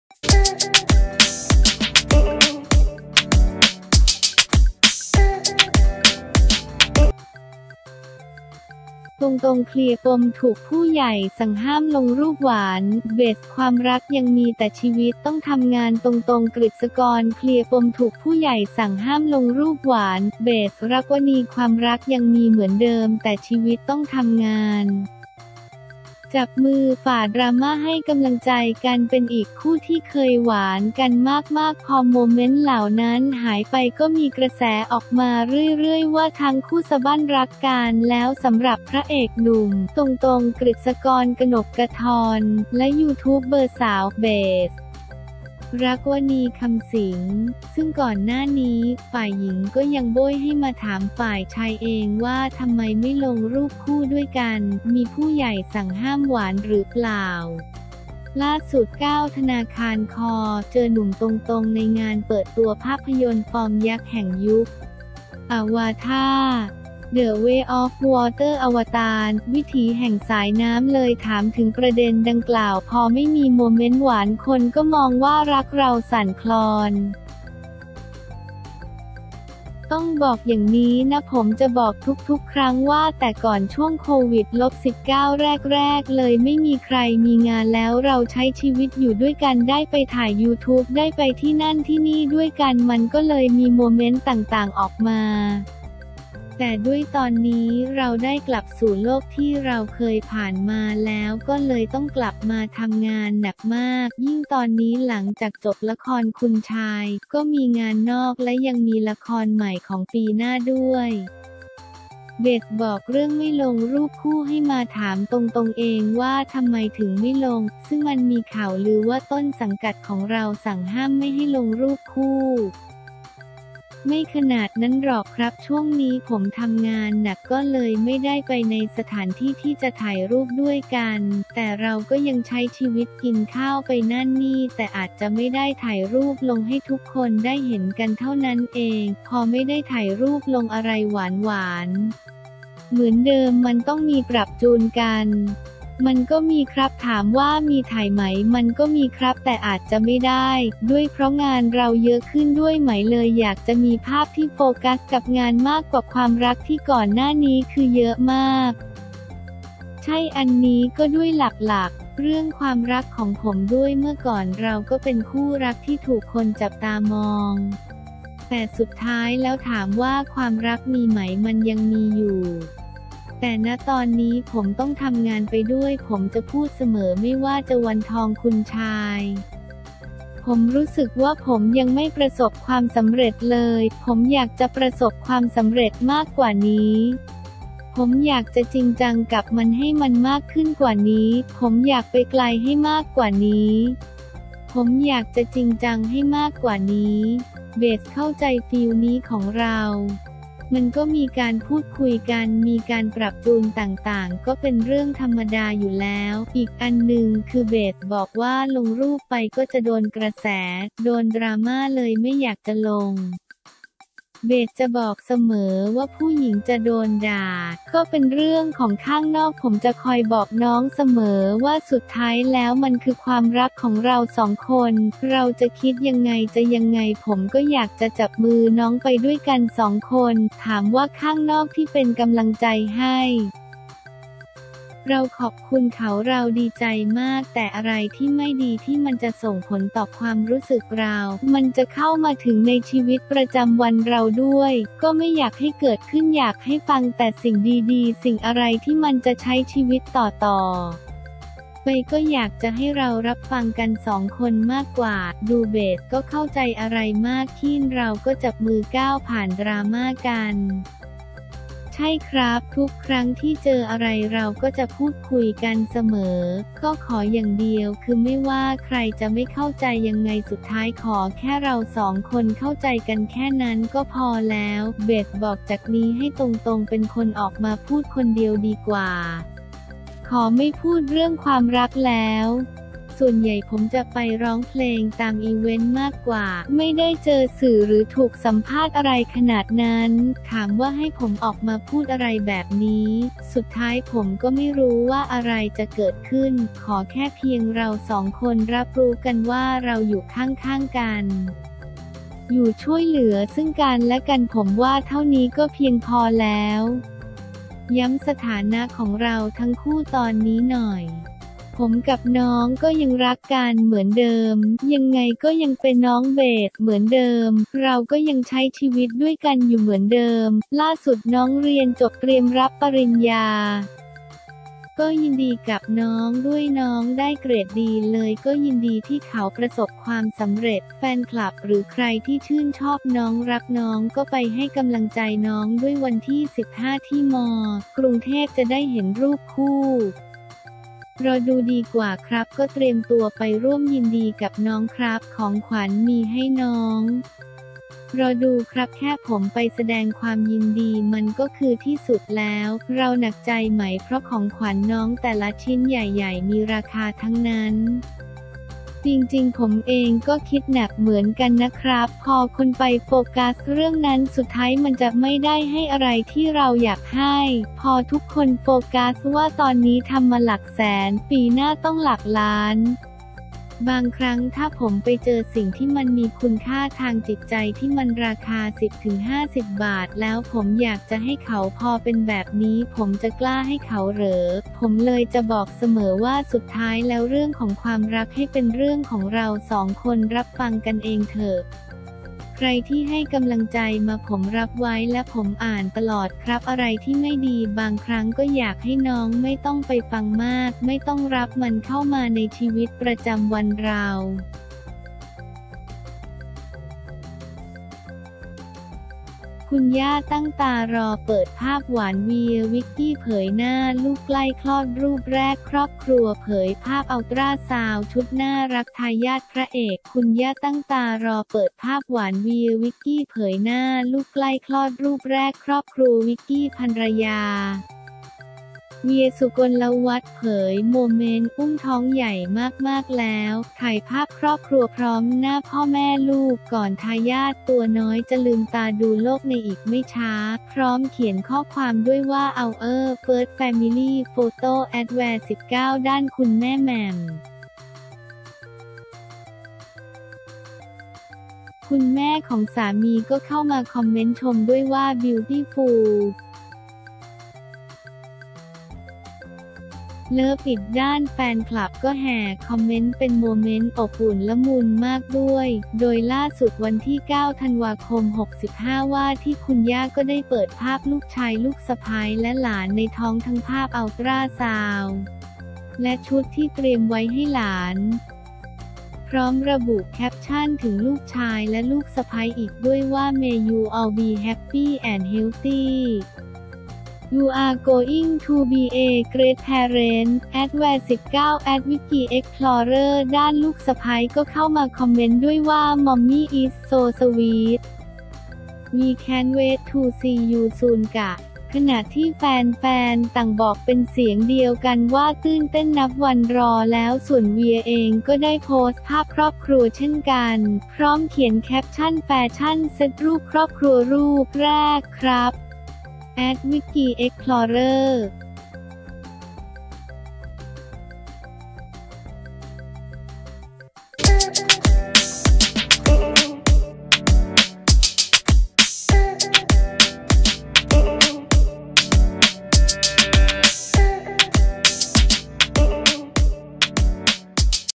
ตงตง เคลียร์ปมถูกผู้ใหญ่สั่งห้ามลงรูปหวาน เบส ความรักยังมี แต่ชีวิตต้องทำงาน
ตงตง กฤษกร เคลียร์ปมถูกผู้ใหญ่สั่งห้ามลงรูปหวาน เบส รักษ์วนีย์ ความรักยังมีเหมือนเดิม แต่ชีวิตต้องทำงาน จับมือฝ่าดราม่าให้กำลังใจกัน
เป็นอีกคู่ที่เคยหวานกันมากๆ พอโมเมนต์เหล่านั้นหายไป ก็มีกระแสออกมาเรื่อย ๆ ว่าทั้งคู่สะบั้นรักกันแล้ว สำหรับ พระเอกหนุ่ม “ตงตง กฤษกร กนกธร” และยูทูบเบอร์สาว “เบส รักษ์วนีย์ คำสิงห์
ซึ่งก่อนหน้านี้ ฝ่ายหญิง ก็ยังโบ้ยให้มาถามฝ่ายชายเองว่าทำไมไม่ลงรูปคู่ด้วยกัน มีผู้ใหญ่สั่งห้ามหวานหรือเปล่า ล่าสุด (9 ธ.ค) เจอ หนุ่มตงตง ในงานเปิดตัวภาพยนตร์ฟอร์มยักษ์แห่งยุค Avatar: The Way of Water อวตาร: วิถีแห่งสายน้ำ เลยถามถึงประเด็นดังกล่าว

พอไม่มีโมเมนต์หวานคนก็มองว่ารักเราสั่นคลอน? “ต้องบอกอย่างนี้นะ ผมจะบอกทุก ๆ ครั้งว่าแต่ก่อนช่วงโควิด-19 แรก ๆ เลย ไม่มีใครมีงาน แล้วเราใช้ชีวิตอยู่ด้วยกัน ได้ไปถ่ายยูทูบ ได้ไปที่นั่นที่นี่ด้วยกัน มันก็เลยมีโมเมนต์ต่าง ๆ ออกมา แต่ด้วยตอนนี้เราได้กลับสู่โลกที่เราเคยผ่านมาแล้วก็เลยต้องกลับมาทำงานหนักมาก ยิ่งตอนนี้หลังจากจบละครคุณชาย ก็มีงานนอก และยังมีละครใหม่ของปีหน้าด้วย”
เบสบอกเรื่องไม่ลงรูปคู่ให้มาถามตงตงเองว่าทำไมถึงไม่ลง ซึ่งมันมีข่าวลือว่าต้นสังกัดของเราสั่งห้ามไม่ให้ลงรูปคู่? “ไม่ขนาดนั้นหรอกครับ ช่วงนี้ผมทำงานหนัก ก็เลยไม่ได้ไปในสถานที่ที่จะถ่ายรูปด้วยกัน แต่เราก็ยังใช้ชีวิต กินข้าว ไปนั่นนี่แต่อาจจะไม่ได้ถ่ายรูปลงให้ทุกคนได้เห็นกันเท่านั้นเอง”
พอไม่ได้ถ่ายรูปลงอะไรหวาน ๆ เหมือนเดิมมันต้องมีปรับจูนกัน? “มันก็มีครับ ถามว่ามีถ่ายไหมมันก็มีครับแต่อาจจะไม่ได้”

ด้วยเพราะงานเราเยอะขึ้นด้วยไหมเลยอยากจะมีภาพที่โฟกัสกับงานมากกว่าความรักที่ก่อนหน้านี้คือเยอะมาก? “ใช่ อันนี้ก็ด้วย หลัก ๆ เรื่องความรักของผมด้วยเมื่อก่อนเราก็เป็นคู่รักที่ถูกคนจับตามอง แต่สุดท้ายแล้ว ถามว่าความรักมีไหมมันยังมีอยู่ แต่ ณ ตอนนี้ผมต้องทำงานไปด้วย ผมจะพูดเสมอไม่ว่าจะวันทอง คุณชาย ผมรู้สึกว่าผมยังไม่ประสบความสำเร็จเลย ผมอยากจะประสบความสำเร็จมากกว่านี้ ผมอยากจะจริงจังกับมันให้มันมากขึ้นกว่านี้ ผมอยากไปไกลให้มากกว่านี้ ผมอยากจะจริงจังให้มากกว่านี้”
เบสเข้าใจฟีลนี้ของเรา? “มันก็มีการพูดคุยกัน มีการปรับจูนต่างๆ ก็เป็นเรื่องธรรมดาอยู่แล้ว”